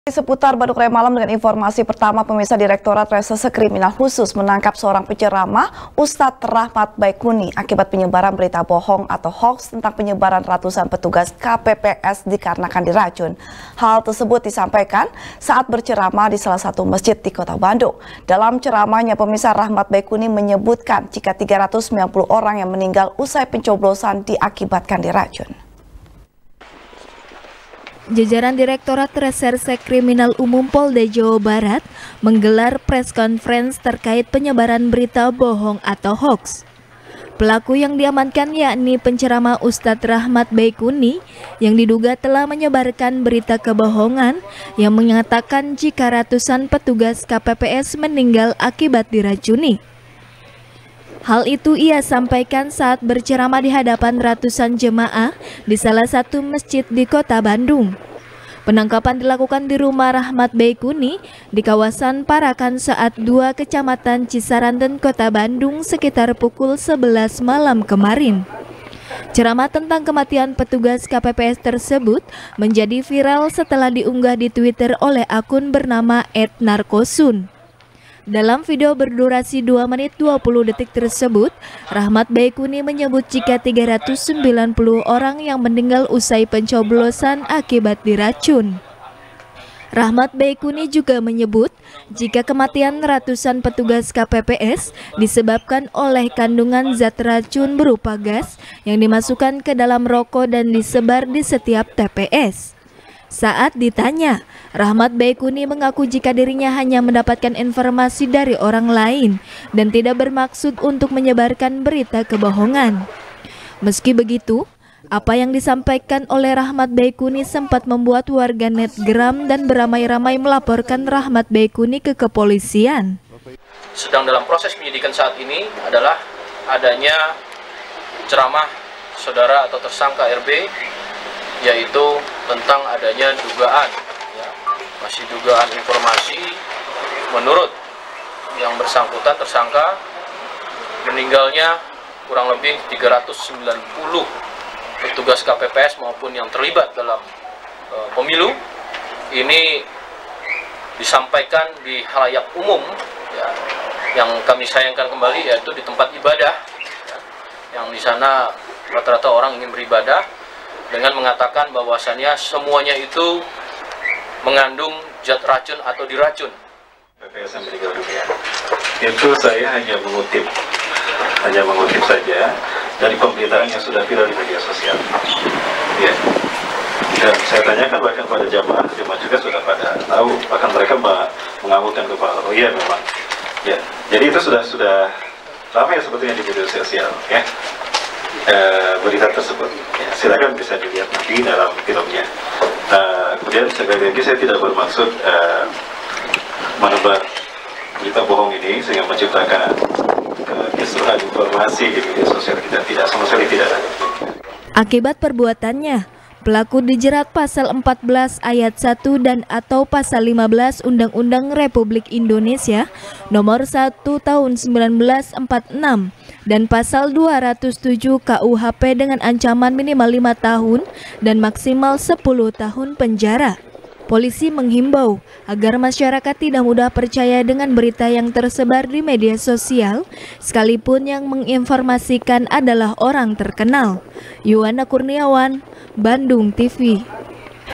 POLDA JABAR Tangkap Ustadz Rahmat Baequni Kasus Penyebaran Berita Hoax | SBR | BANDUNGTV
SBR | BANDUNGTV | FARHAT MUMTAZ | Direktorat Reserse Kriminal Khusus, menangkap seorang penceramah, Ustad Rahmat Baequni, akibat penyebaran berita bohong atau hoaks, tentang kematian ratusan petugas K-P-P-S karena diracun. Hal itu ia sampaikan saat berceramah, di salah satu masjid di kota Bandung. Dalam ceramahnya, Rahmat Baequni menyebut jika 390 orang yang meninggal usai pencoblosan, akibat diracun.